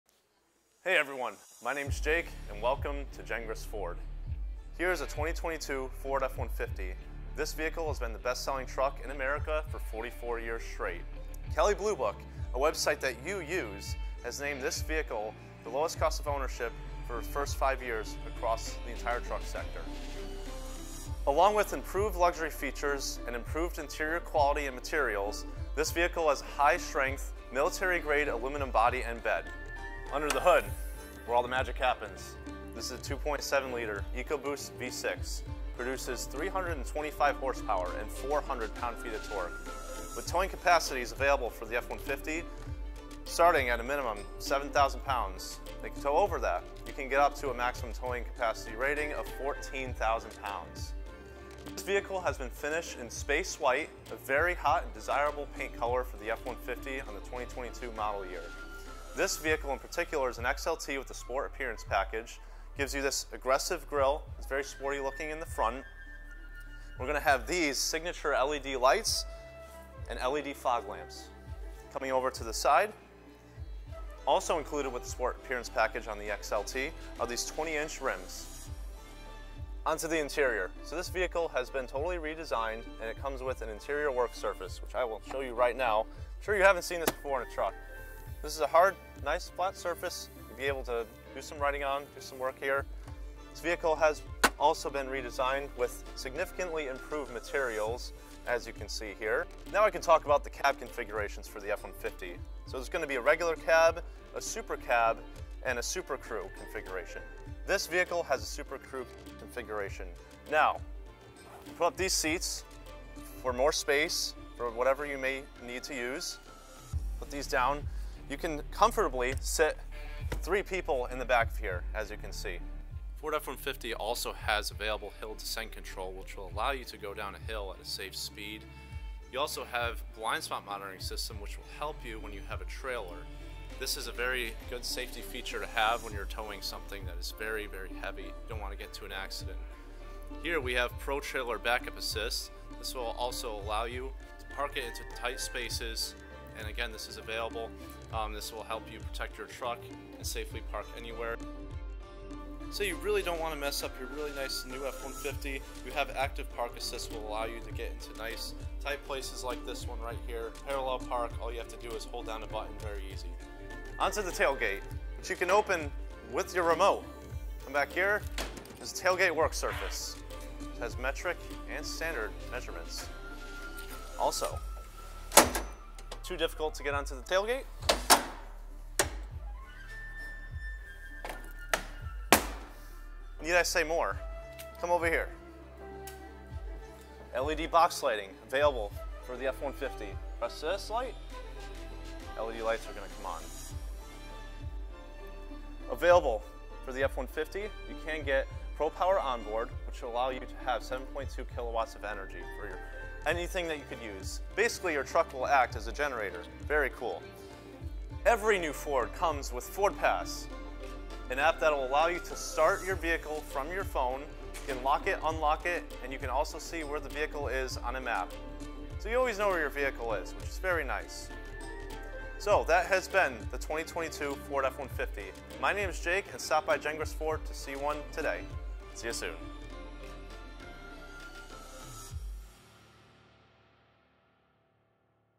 2022 Ford F150 Full Review | Interior Features | Exterior | Towing Capacity | Payload | Ford Truck
Ford's F-150 has been the best selling truck in America for 44 years in a row, and the best selling vehicle in America for 39 years in row. The 2022 Model of this American Icon will surely be no different. With amazing performance such as best in class towing and payload, a high strength military grade aluminum body and bed, and a redesigned mobile office interior, the 2022 Ford F-150 might just be the best pickup ever.

We're located at 225 New Britain Ave, Plainville, CT
or visit us online at gengrasford.com